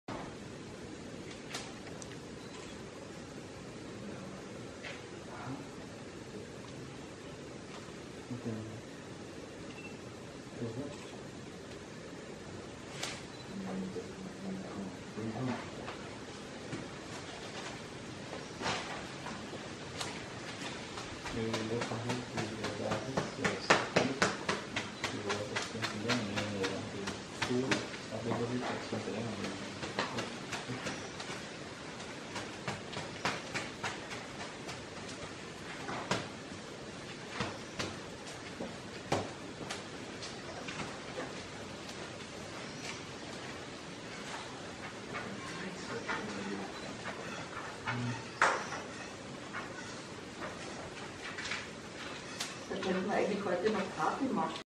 cassava bread made by a Makuski Indian - Surama Village - Guyana
A Makushi Indian is preparing the cassava bread on an oven in a house outside of Surama Village - Northern Rupununi - Guyana